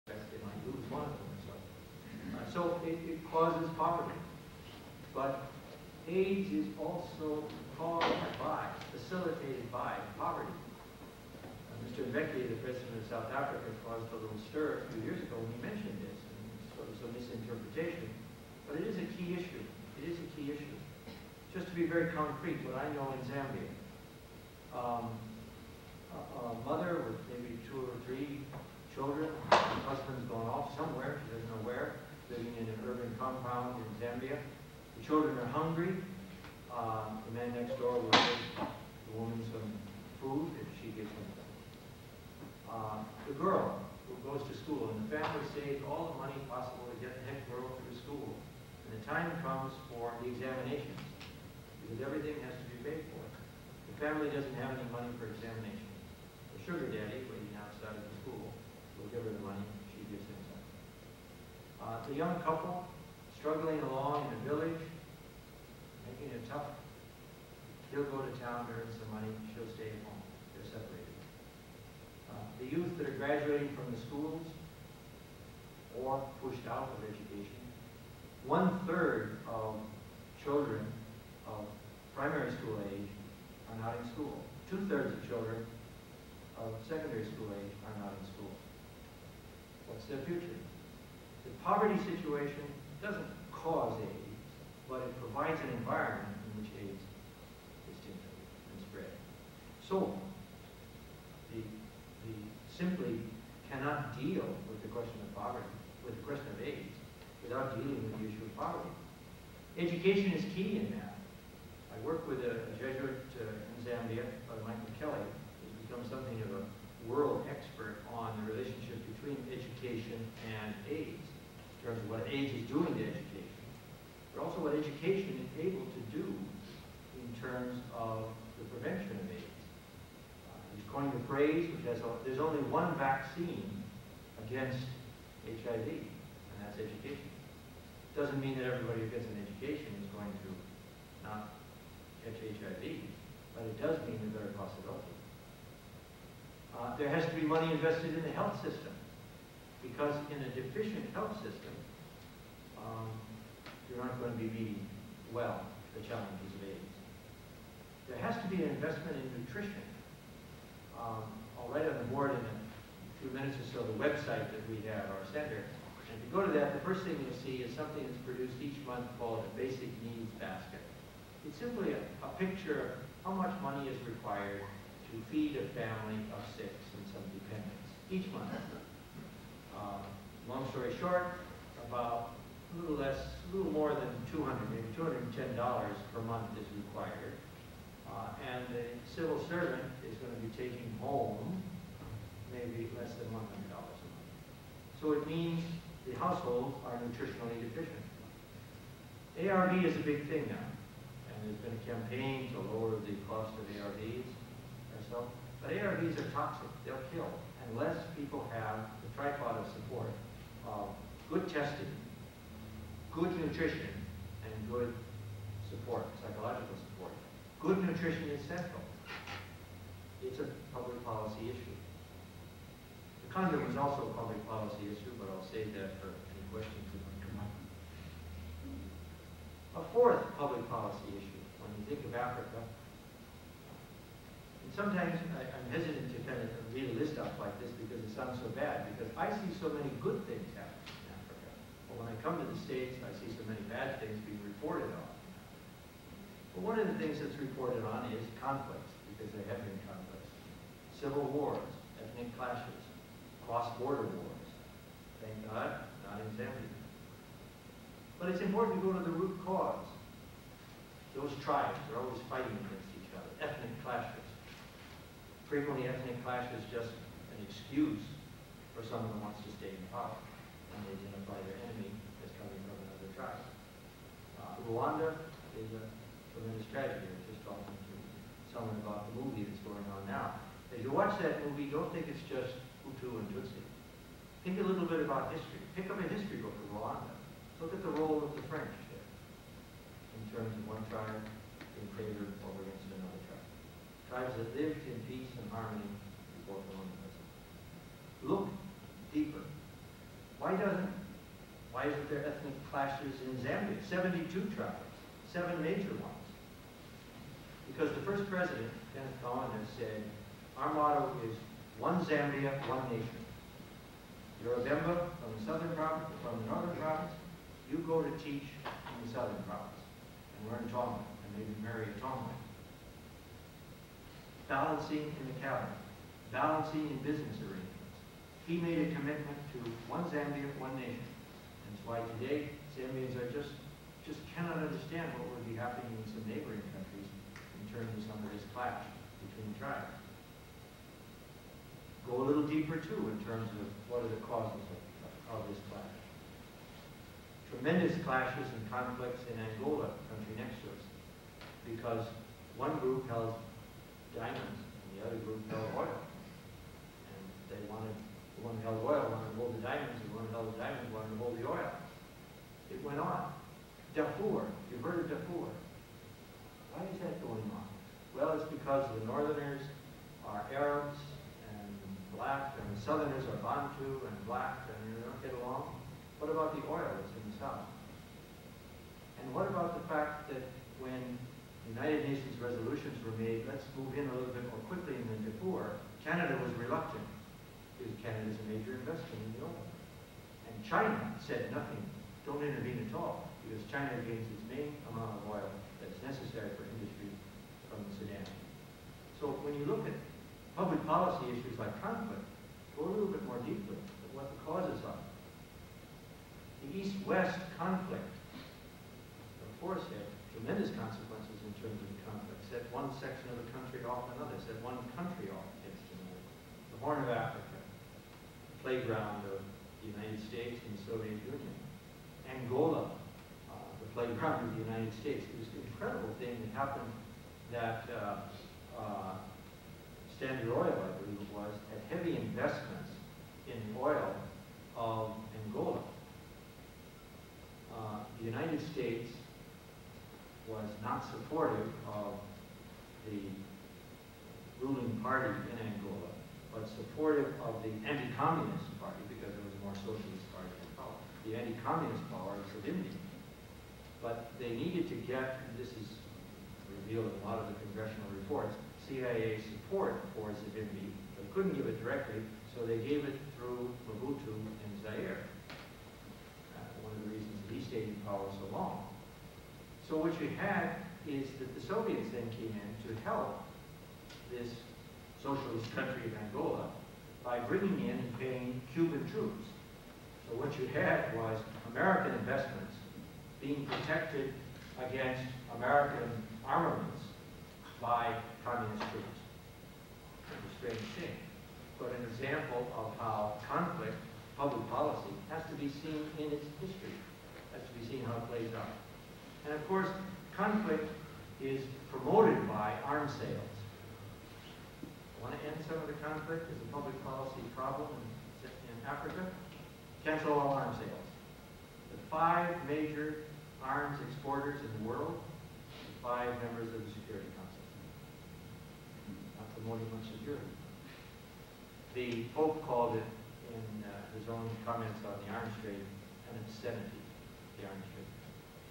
Peter Henriot, SJ, Jesuit Centre for Theological Reflection, Zambia (3 of 8)
Catholic Social Thought and African Public Policy
Peter Henriot, SJ, Jesuit Centre for Theological Reflection, Zambia